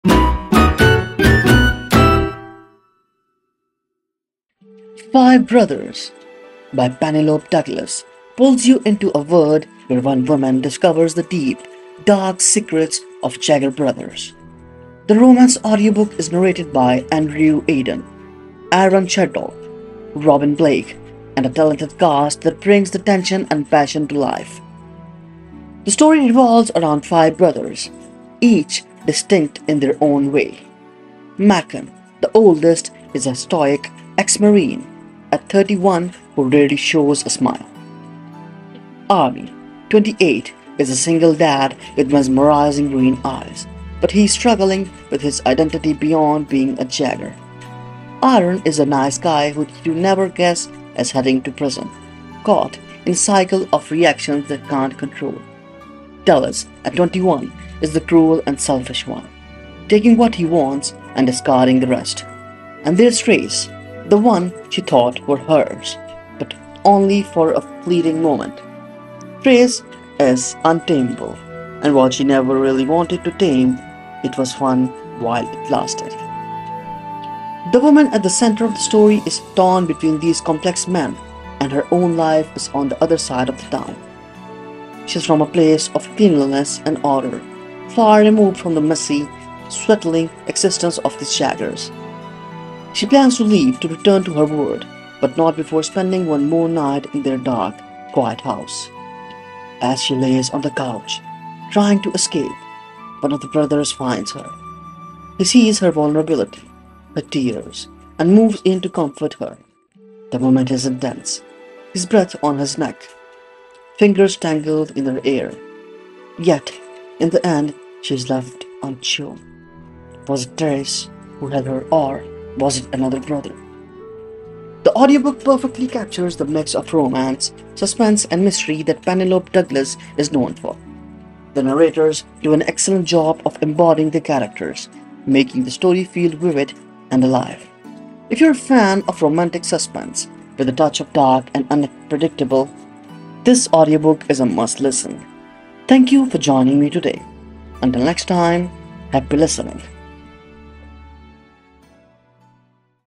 Five Brothers Audiobook Review | Penelope Douglas Audiobook | BookWorms
Check out my review of Five Brothers Audiobook by Penelope Douglas, the latest romantic suspense that’s got everyone talking! In this video, I dive deep into the story of the Jaeger brothers and the secrets they hold. Each brother has a unique personality—Macon, the serious ex-Marine; Army, the struggling single dad; Iron, the nice guy headed for prison; Dallas, the cruel and selfish younger brother; and Trace, the wild and untamable one.

The Five Brothers Audiobook is narrated by a fantastic cast including Andrew Eiden, Aaron Shedlock, and Robin Blake, bringing the intensity and passion of the story to life. If you love romance with a dark twist and complex characters, you won't want to miss this audiobook.

Share your thoughts on Five Brothers Audiobook in the comments below!